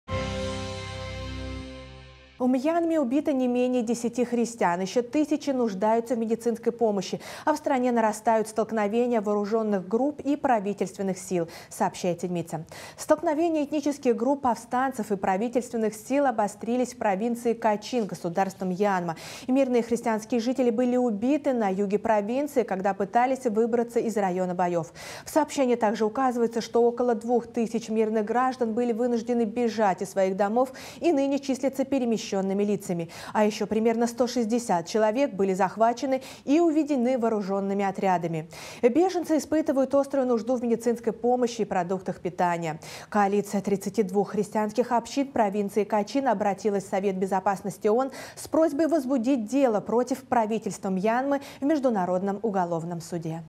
Убийство мирных жителей
В Мьянме тысячи христиан нуждаются в неотложной медицинской помощи. О том, каким гонениям подвергаются христиане в этом государстве и почему, вы узнаете в сюжете.